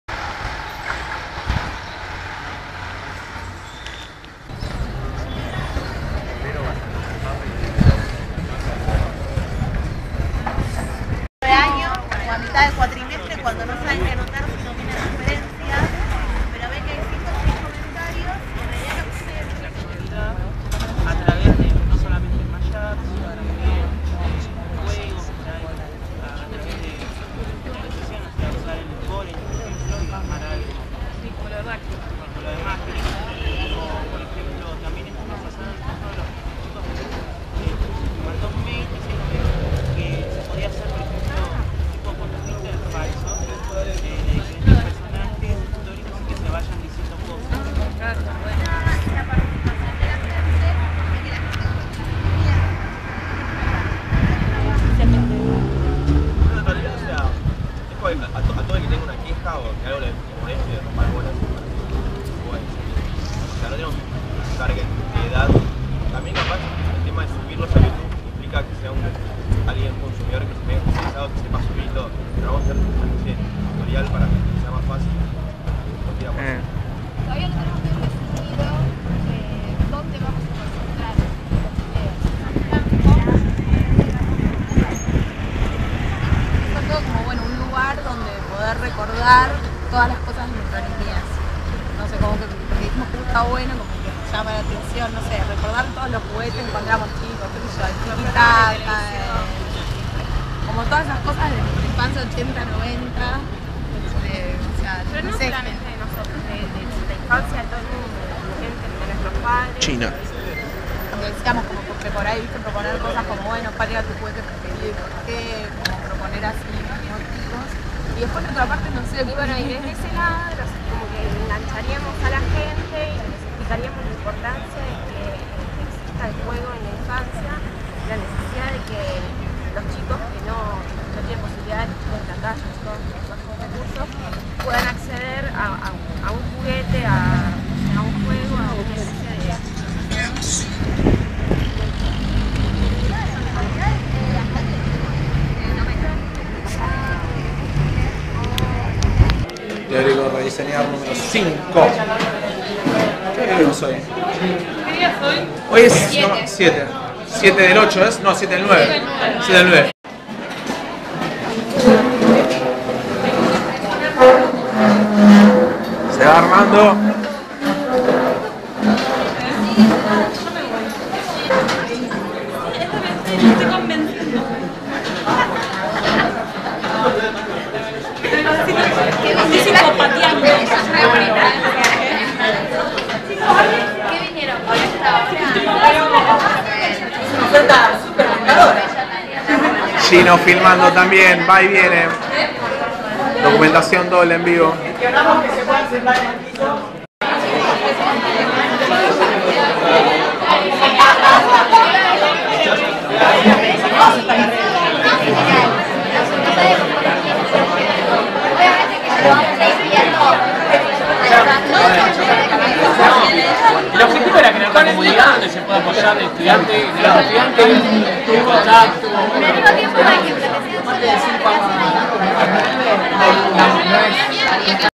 Clases Públicas en fsoc
Comisión 11 a cargo de Caro Gruffat, Gino Cingolani, Ceci Sabatino Arias, Sara Bordagreen, Lucas Esteban Delgado y Giselle Bordoy.

Parte del (No) No-Teórico durante la toma, se realizaron consultas de los grupos a sus respectivos ayudantes pero no se pudo dar la clase con normalidad.